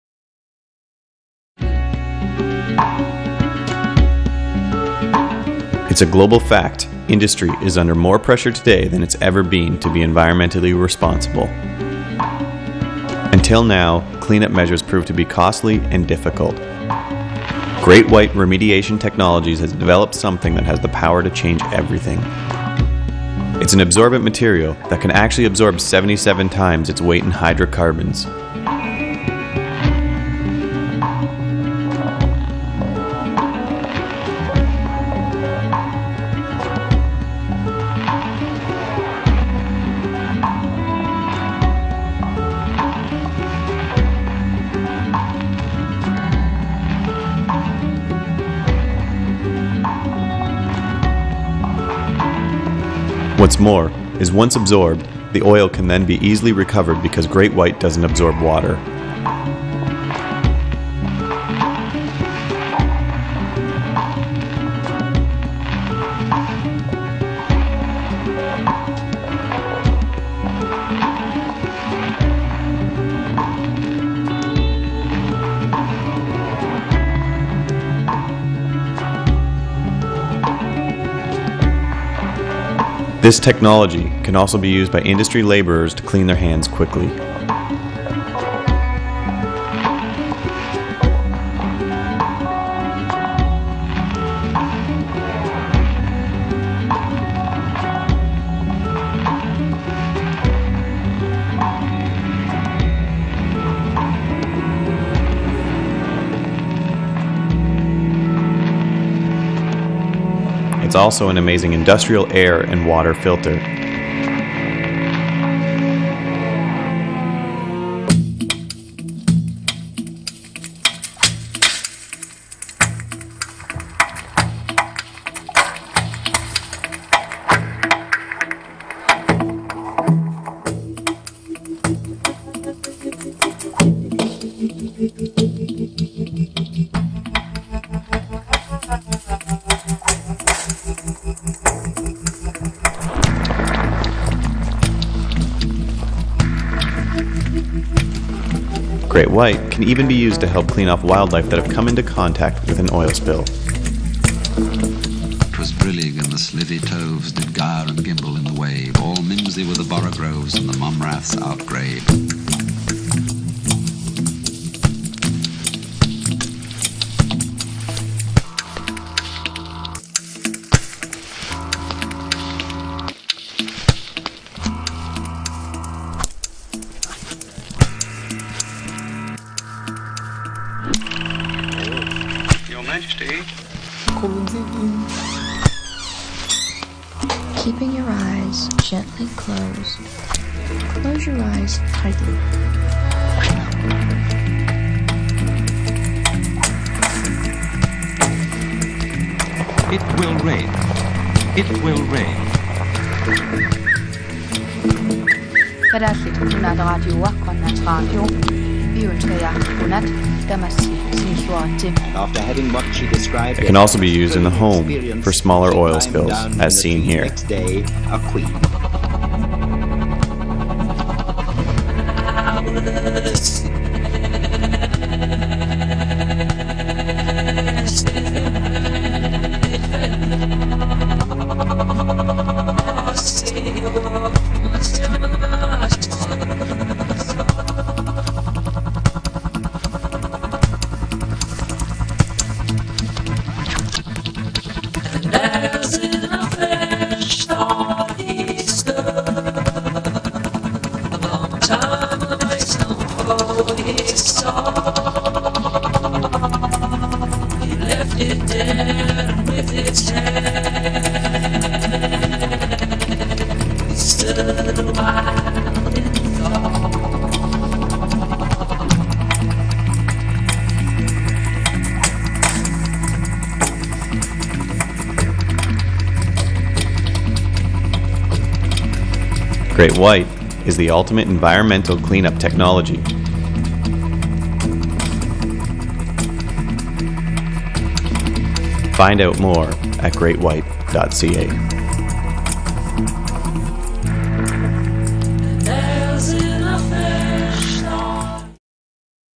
Great white Remediation Technologies
New product from Great White Remediation has a lot of usefull applications.  Oil spill pickup that doesnt pick up water.  Hand cleaning for industry workers. Industrial water and air filter. Used to clean animals that come in contact with oil spills. Can be used for in home small oil spills as well.